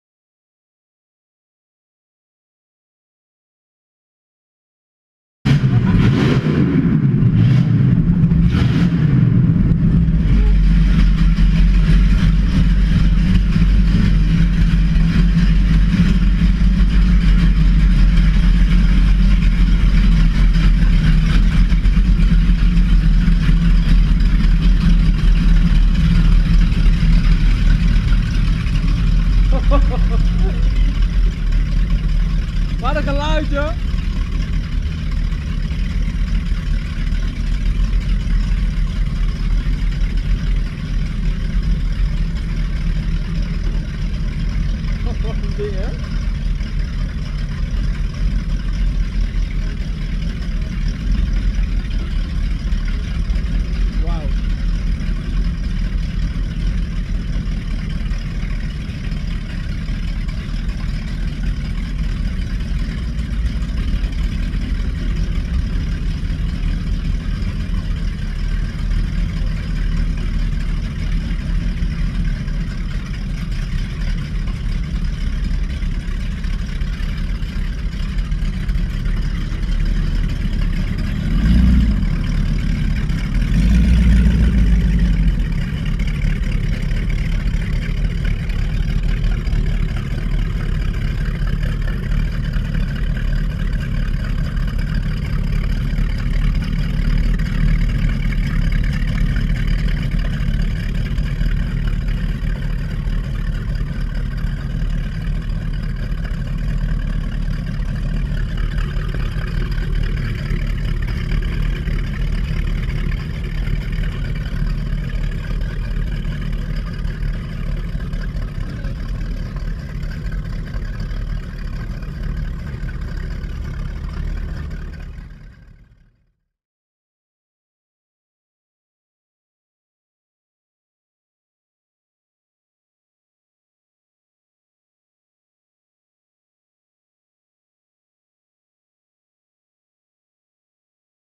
Odyssee - V8
Eine schöne Bootsfahrt mit Twin-V8-Sound.

Danke Wolfgang!

Equipment:

Kamera 1
Kamera 2

Schnitt und Bearbeitung:

Hauptsächlich komplett auf dem iPhone 6s plus, also Kamera 1, mithilfe folgender Apps: